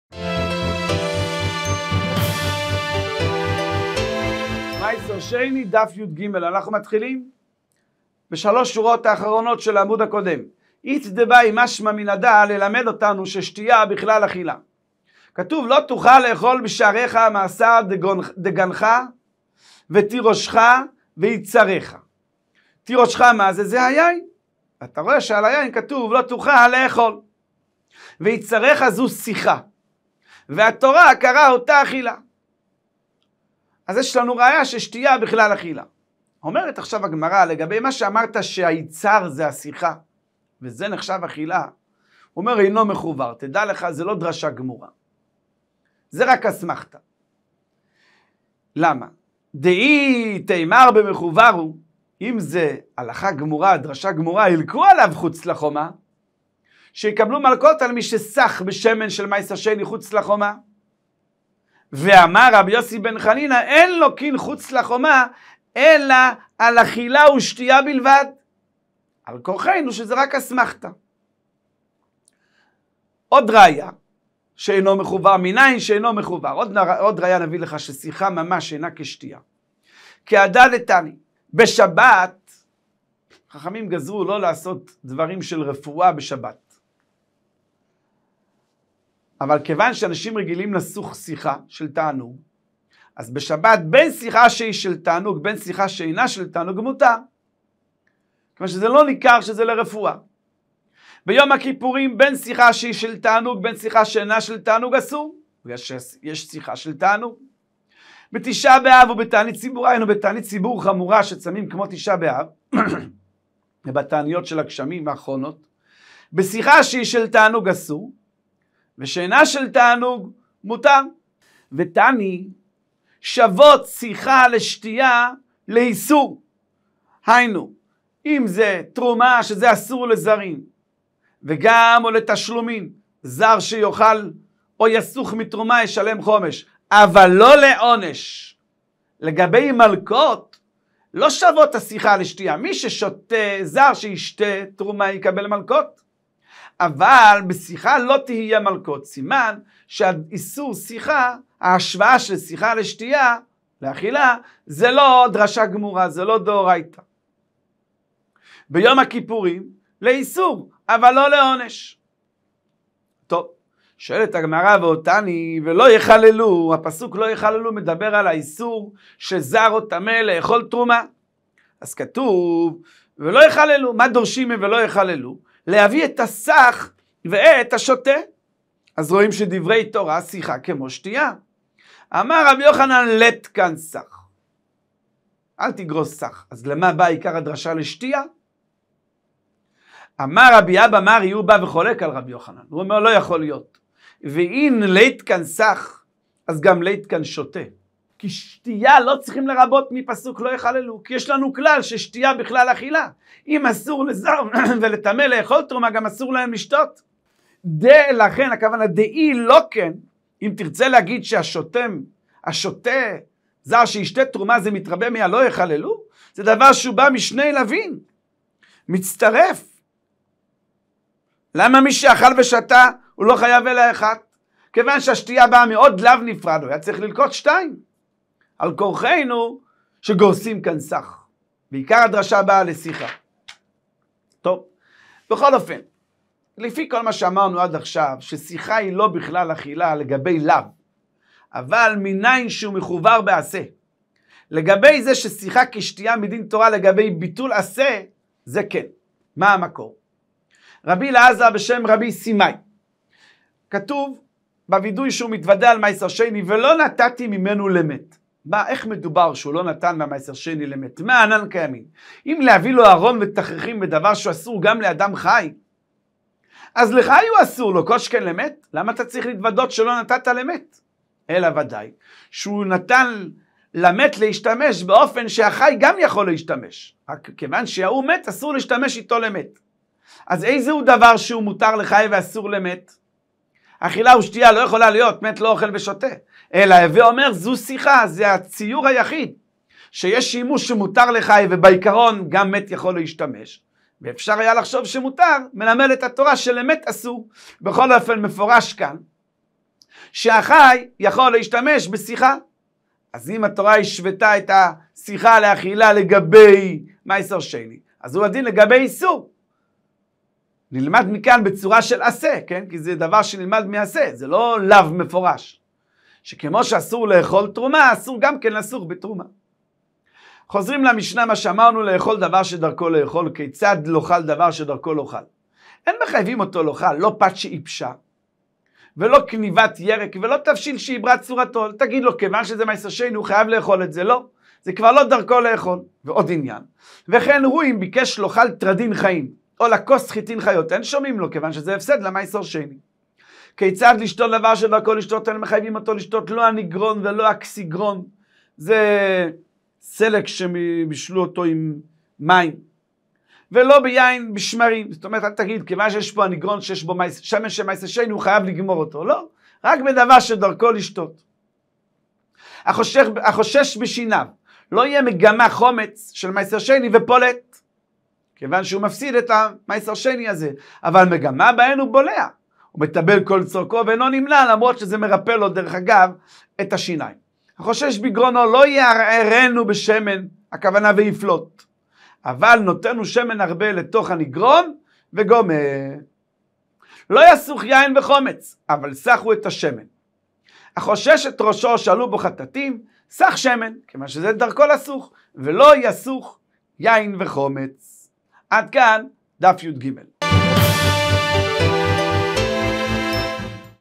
הדף היומי ירושלמי מסכת מעשר שני דף יג
דף היומי - שיעורי הדף היומי בגמרא תלמוד ירושלמי מסכת מעשר שני מבית "דרשו" עם הרב אליהו אורנשטיין שליט"א, הדף יומי קצר וישיבתי